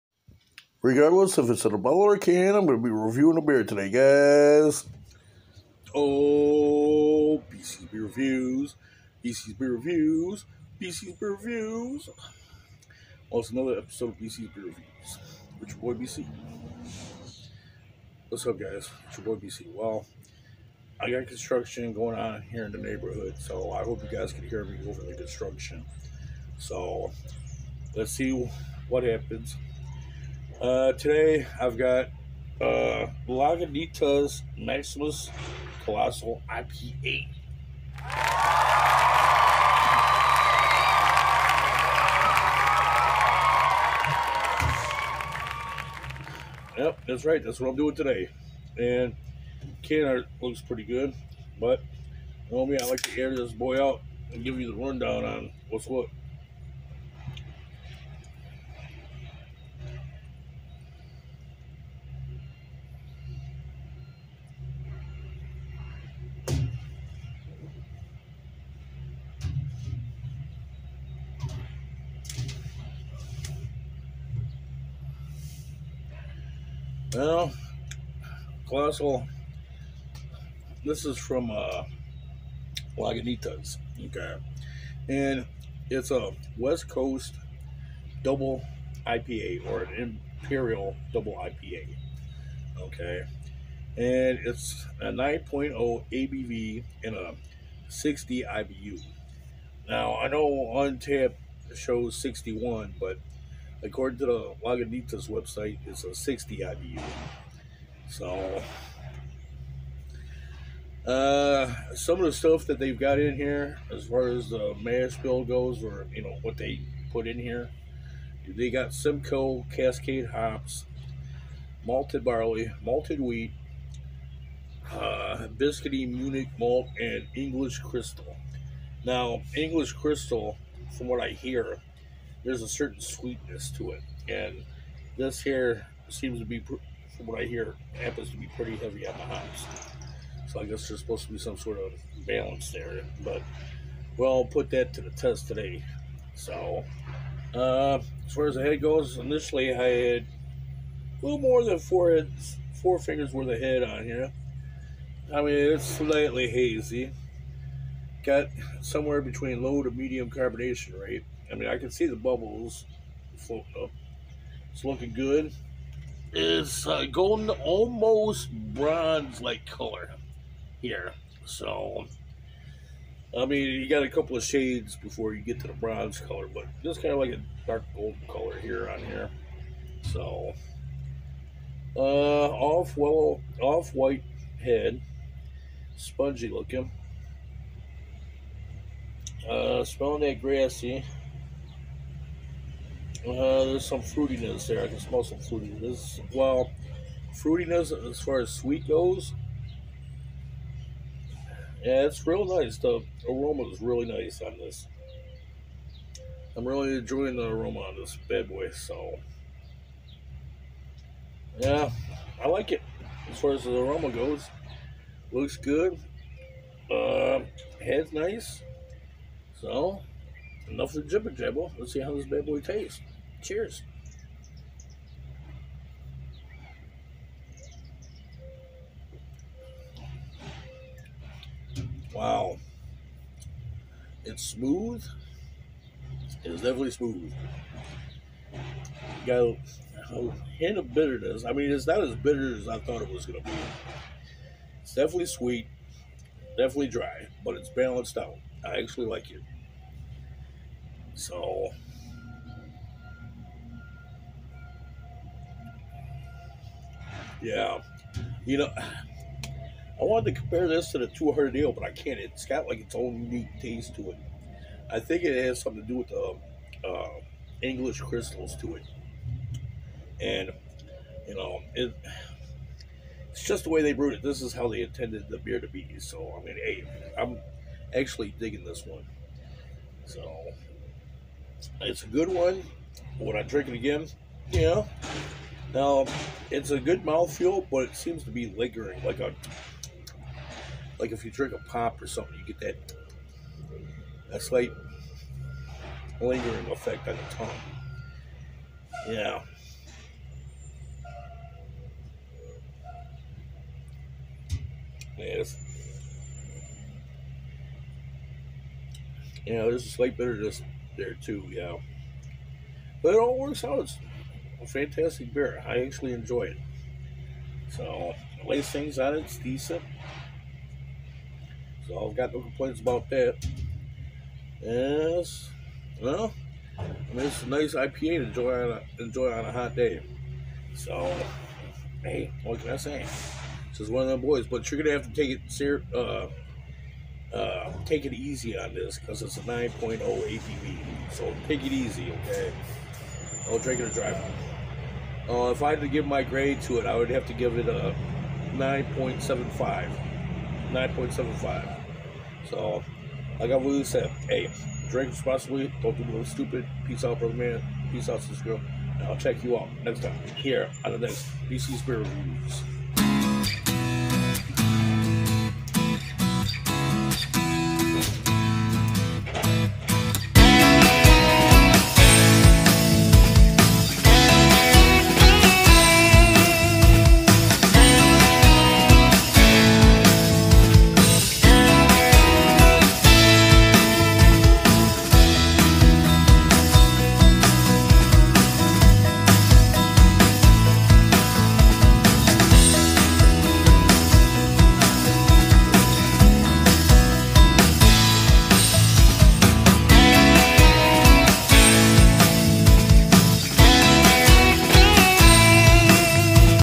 Maximus colossal IPA by lagunitas
lagunitas Maximus colossal IPA is an imperial double IPA is beer comes at you with a 9.0 ABV And a 60 IBU now this is a pretty good balance IPA getting a lots of hoppiness and citra and some sweetness the floral aroma is awesome the color is amazing The taste is a phenomenal